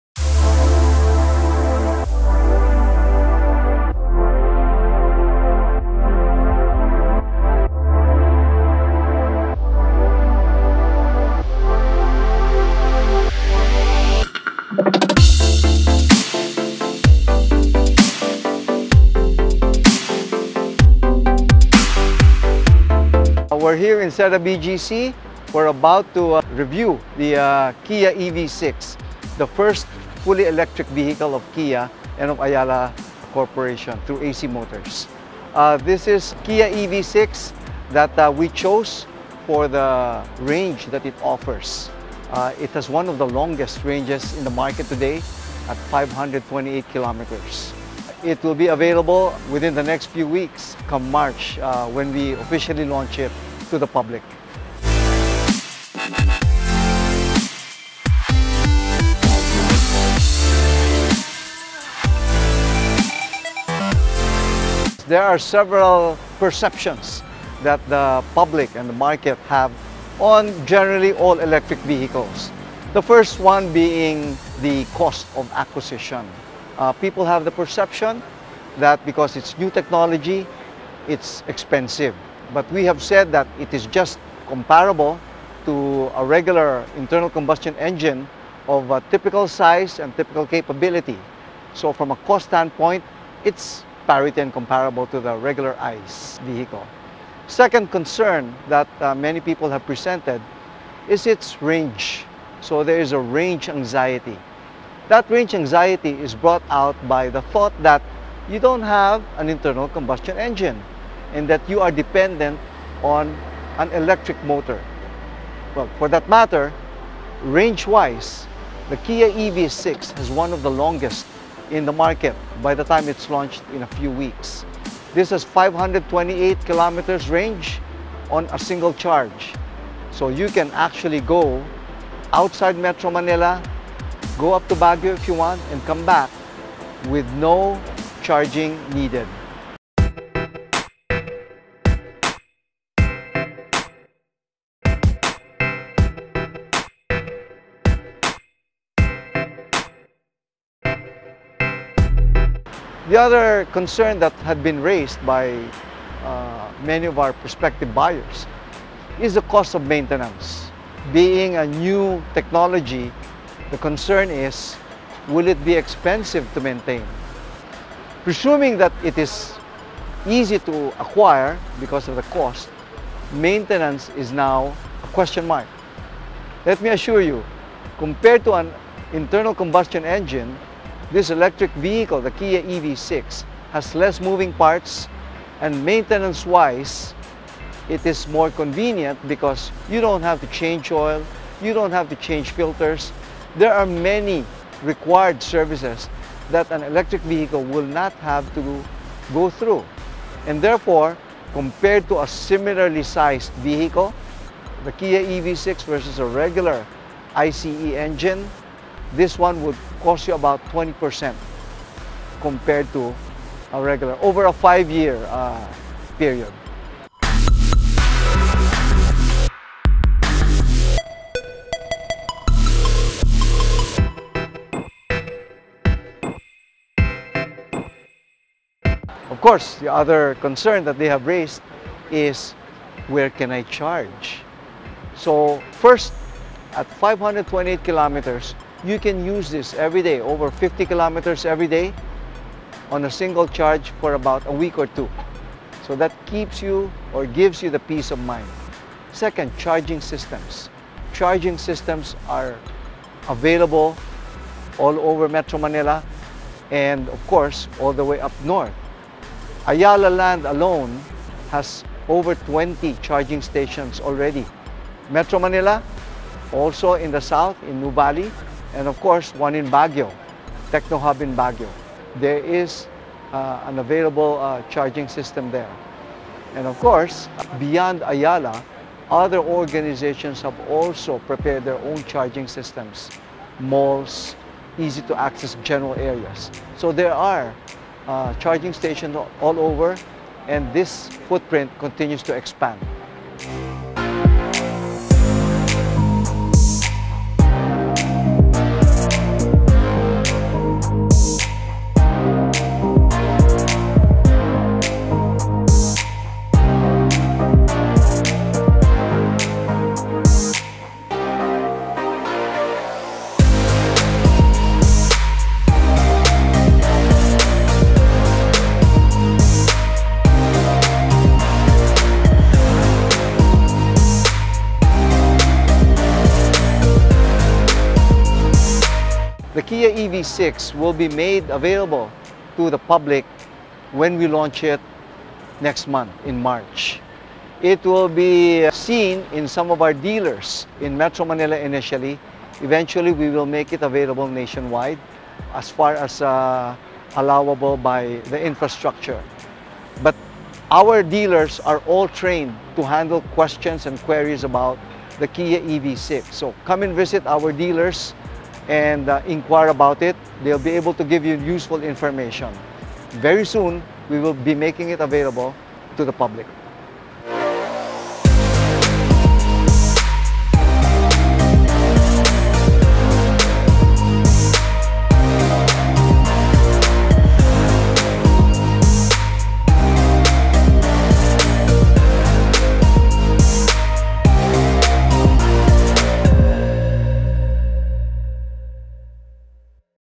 2023 Kia EV6 GT Line
Kia Philippines gave a preview and a first hand feel of the soon to be launched Kia EV6. Kia's first entry to the electric vehicle segment.